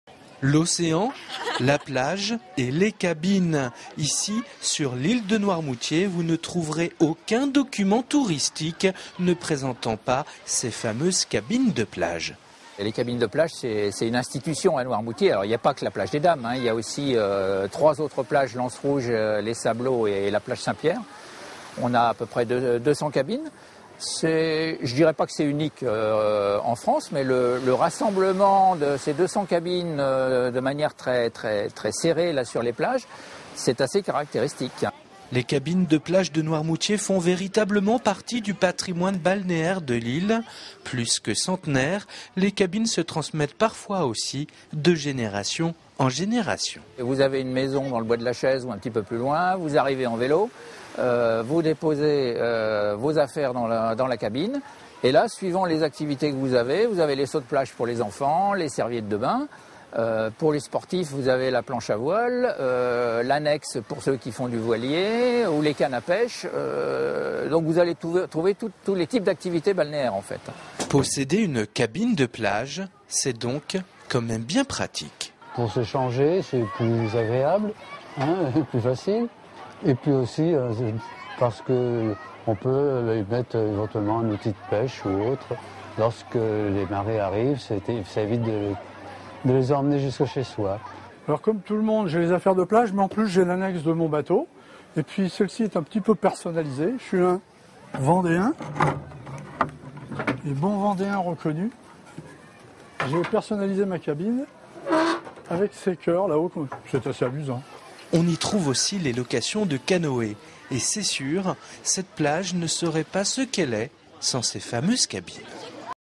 Les cabines de plages de Noirmoutier
Sites webs/blogs, copiez cette vidéo avec le bouton "Intégrer" !
 Balade insolite sur l'Ile de Noirmoutier pour aller à la rencontre des propriétaires des cabines de plages. De petites baraques en boite qui se transmettent parfois de génération en génération.